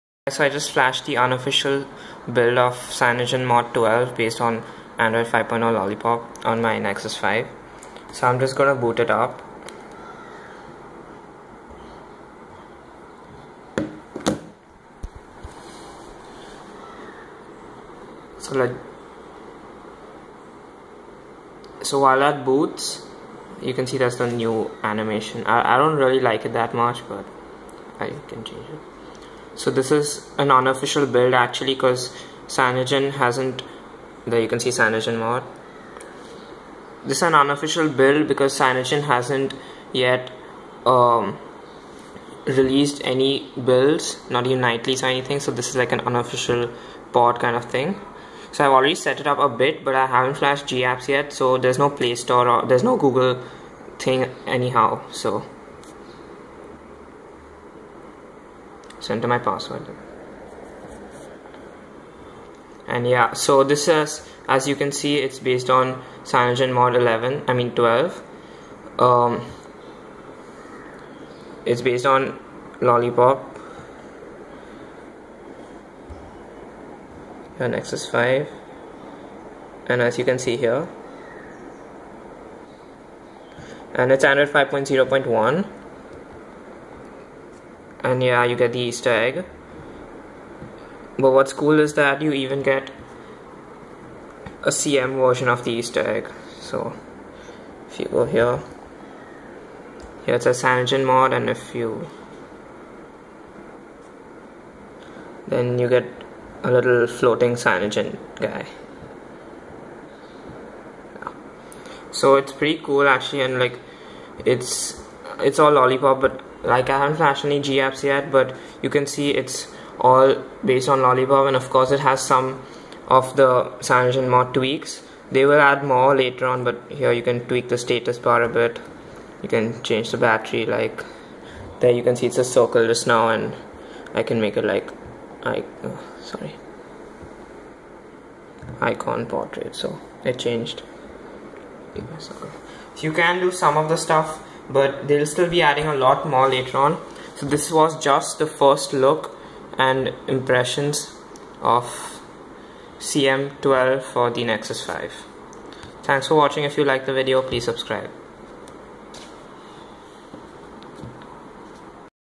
CyanogenMod 12 on Nexus 5: First Look!
I just flashed the unofficial build of CyanogenMod 12 (based on Lollipop 5.0.1) on my Nexus 5, and here's a quick first (actually, second) look of how it works. You must have a custom recovery like TWRP or CWM installed in order to flash the ROM.zip file just like any other ROM.

All instructions and download links can be found in the XDA thread